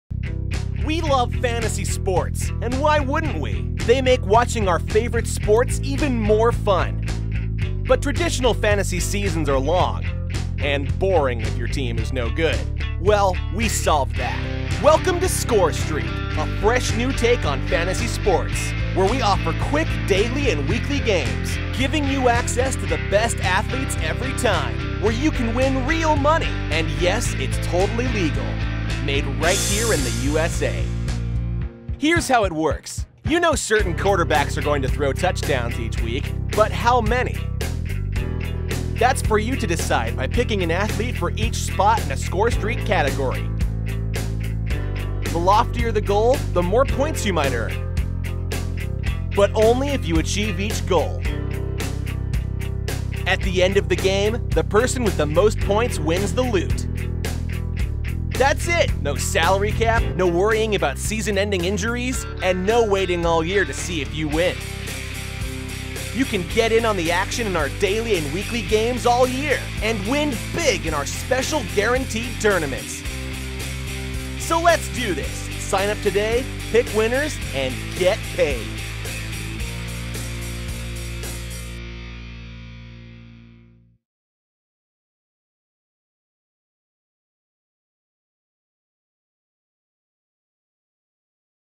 How ScoreStreak Works
How does ScoreStreak work?  This video walks you through what makes ScoreStreak different than traditional daily fantasy sports websites.  We've created a new game that removes the salary cap and lets you pick any player you like, in a straightforward and extremely fun daily fantasy format.